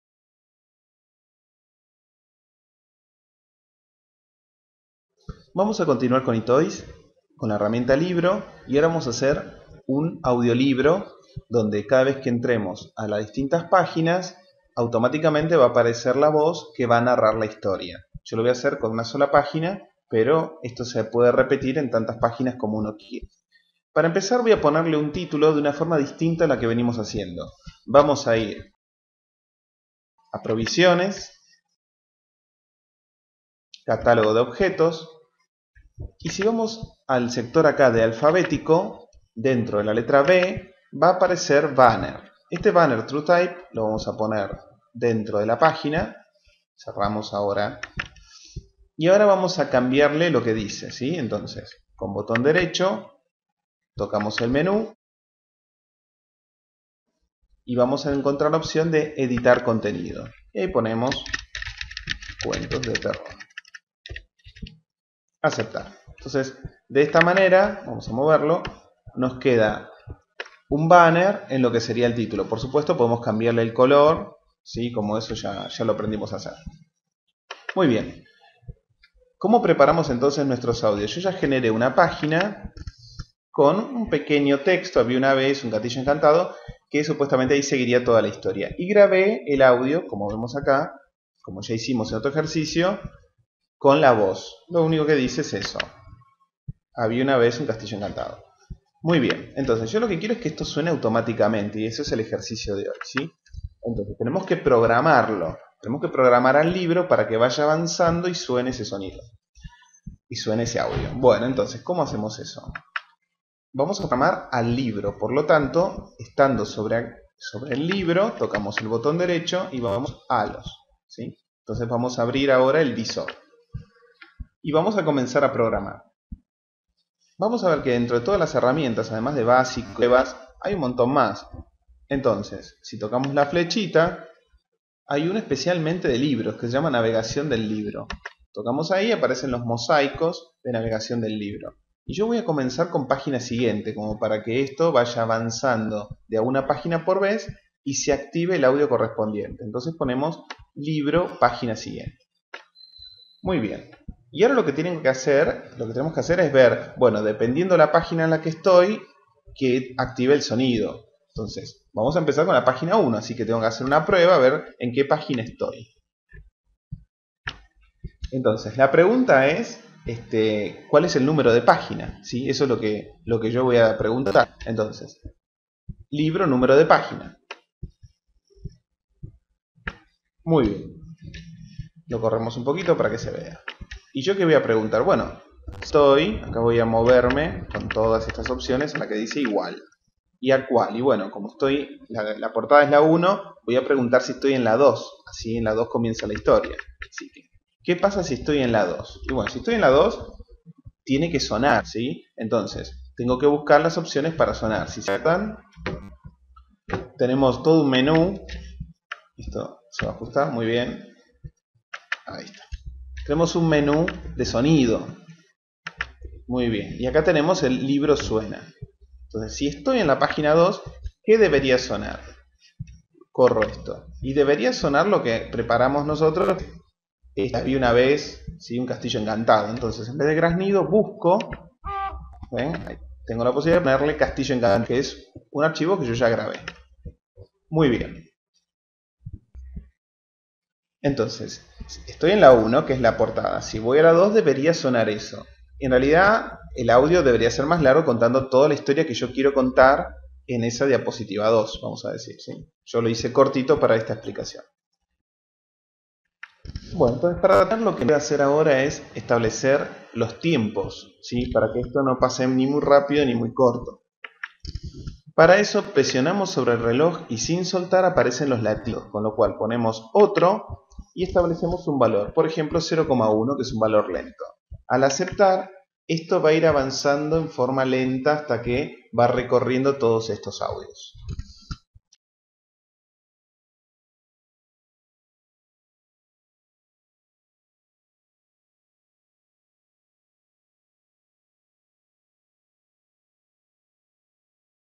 Etoys - Cómo crear un audio libro
Video tutorial que explica cómo crear un audio libro con Etoys.